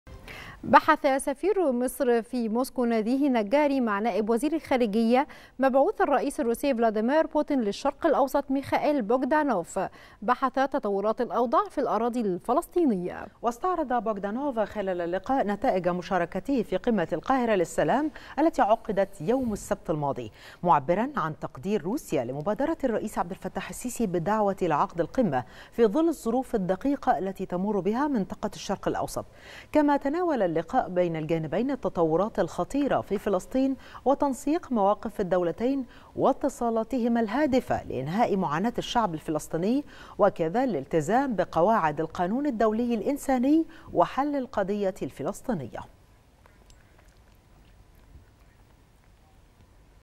سفير مصر في موسكو يبحث تطورات الوضع في الأراضي الفلسطينية مع مبعوث الرئيس الروسي للشرق الأوسط
نشرة الثالثة | سفير مصر في موسكو يبحث تطورات الوضع في الأراضي الفلسطينية مع مبعوث الرئيس الروسي للشرق الأوسط